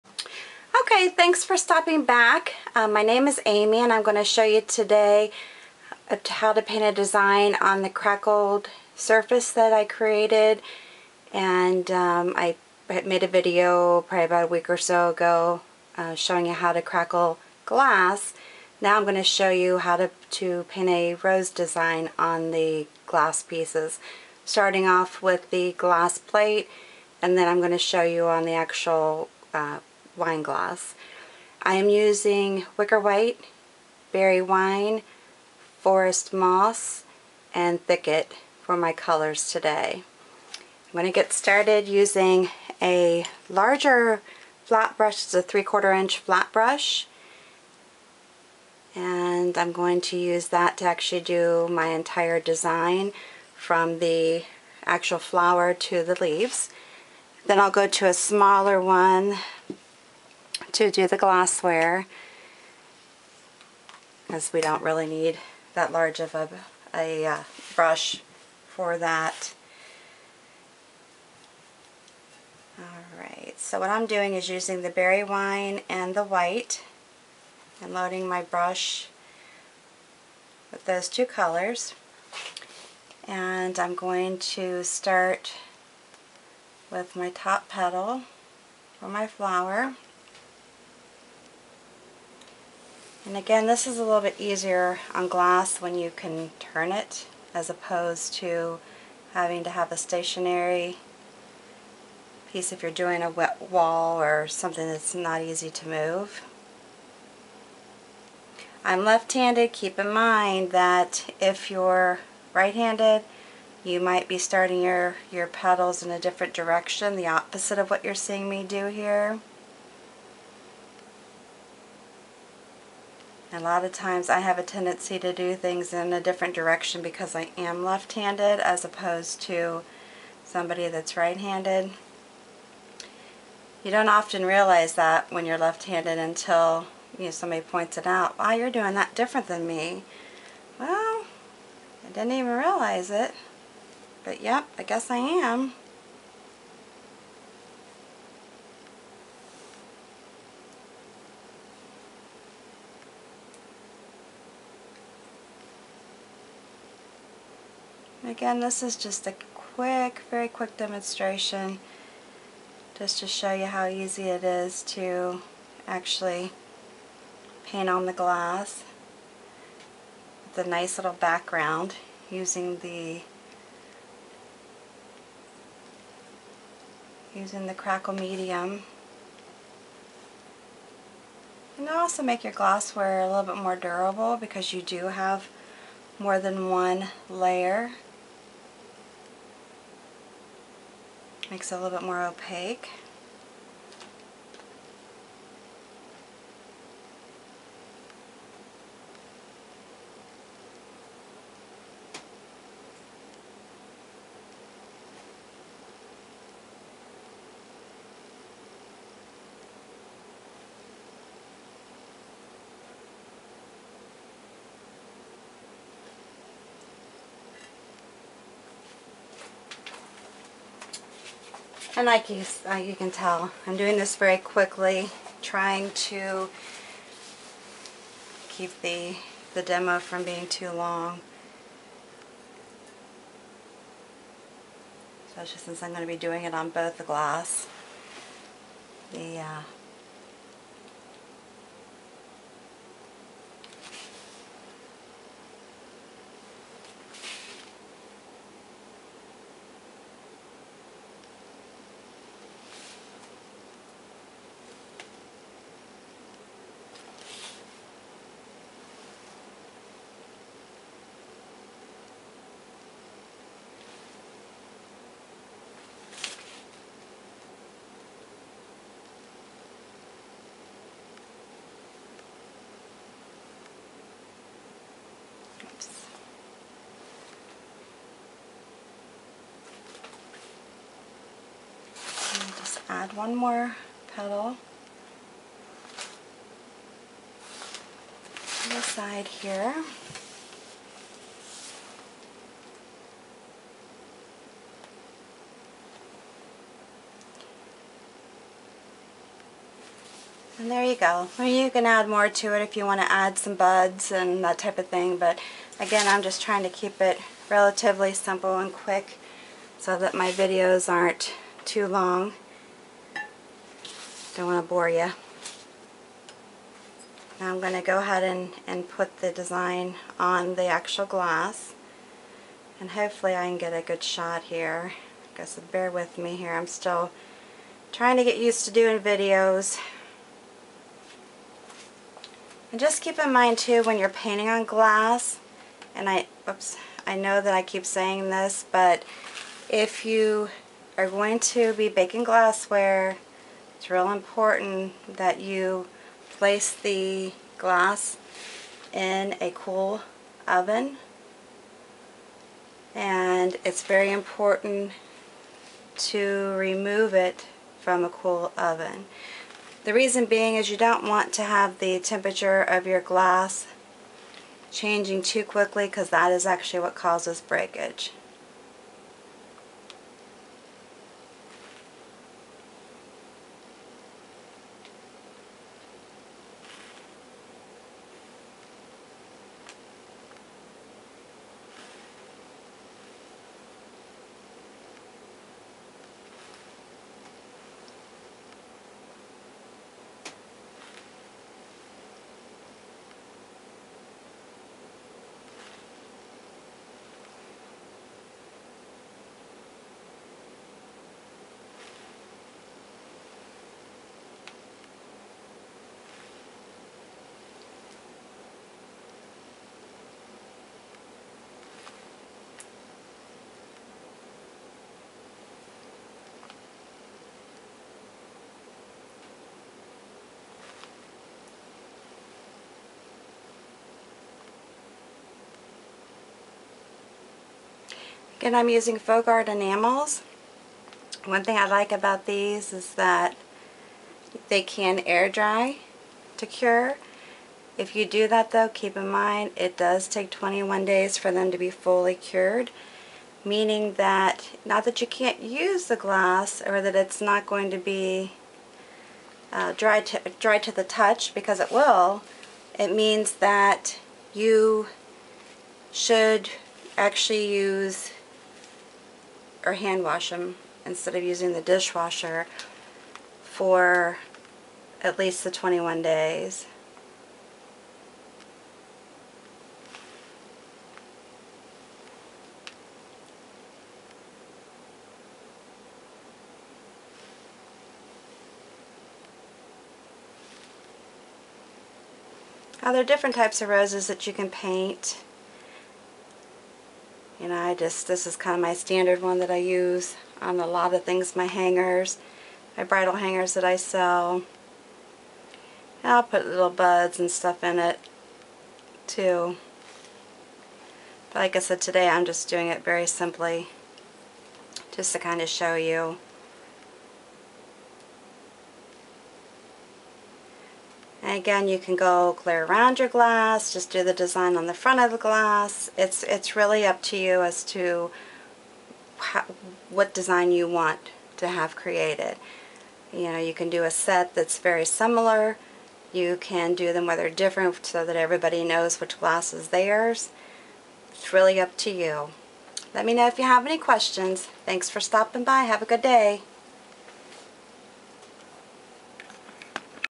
Painting A Rose On a Ivory Crackle Background on a Wine Glass Video
Handing painting a rose on a ivory crackle background on a glass plate and wine glass video. The ivory crack provides a nice background to add the painted design on.

Check back often  for new videos.